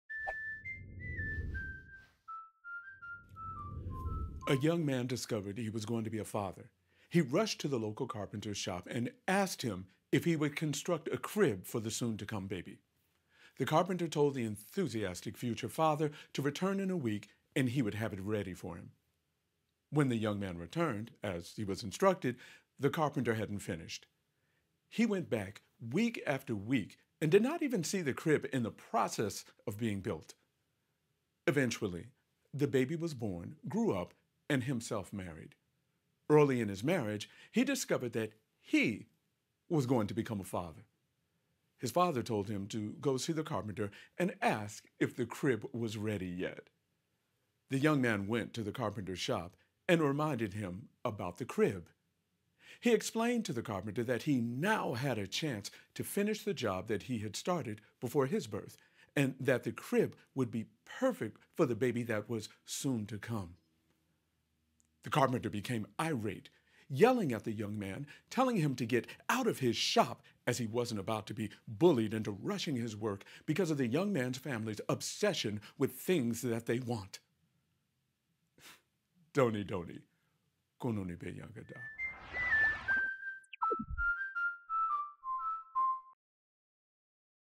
Baba Sharing the Story of a Carpenter's Cognitive Dissonance
In this video Baba the Storyteller is sharing the story of an unreliable carpenter whose cognitive dissonance is a bit on the extreme side. Thumbs up if you enjoy it!